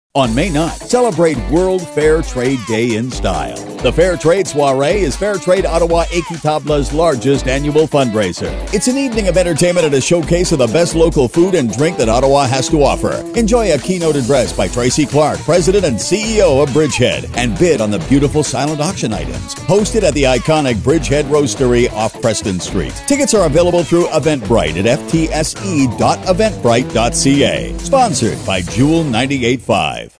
Fair Trade Soirée Équitable 2015: Jewel 98.5 radio spot
The PSA and a slideshow for Fair Trade Ottawa Équitable's 2015 Soirée fundraiser at the Bridgehead Roastery. today! Audio originally appeared on The Jewel 98.5 and is their property. Photographs and final video edit are the property of Fair Trade Ottawa Équitable.